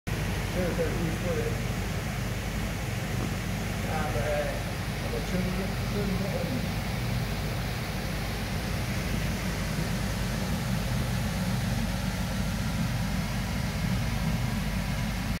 Kikokuryu ki 12 15 cm Kweker: Aoki Geslacht: Leeftijd: Tosai (1 jaar)
In de Vijver Showroom van A2KOI zijn meerdere variëteiten Japanse Koi op voorraad en te koop. A2KOI Dé Japanse Koi Specialist van Nederland & België

Soort Kikokuryu ki
ca: 12  15 cm
Kweker: Aoki
Geslacht:
Leeftijd: Tosai (1 jaar)